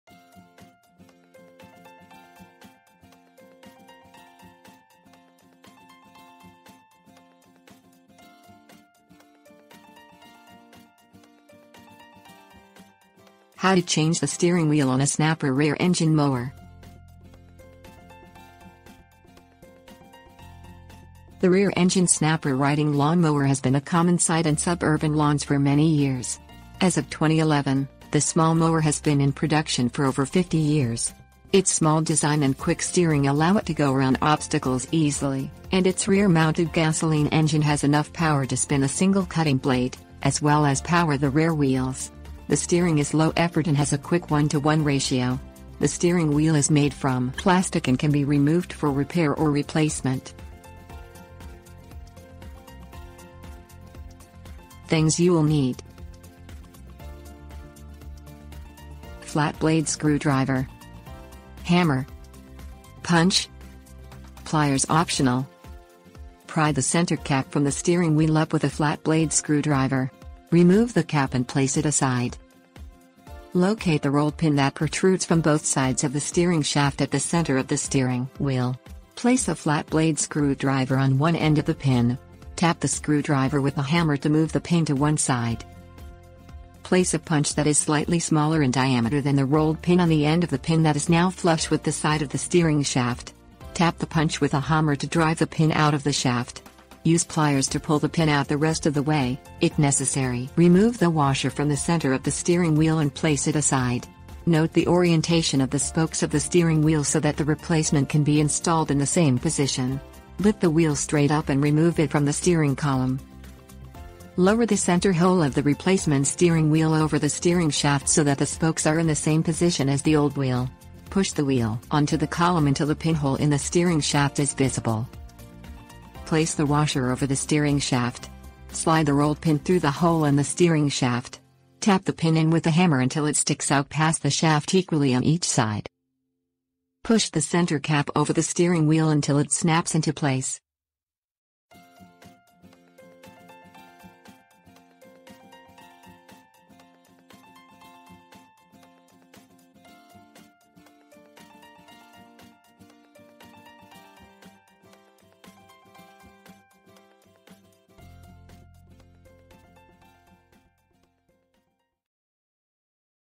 How to Change the Steering Wheel on a Snapper Rear-Engine Mower
How to Change the Steering Wheel on a Snapper Rear-Engine Mower. The rear-engine Snapper riding lawnmower has been a common sight in suburban lawns for many years. As of 2011, the small mower has been in production for over 50 years. Its small design and quick steering allow it to go around obstacles easily, and its rear-mounted gasoline engine has...

Table of contents How to Change the Steering Wheel on a Snapper Rear-Engine Mower
Things You'll Need 00:53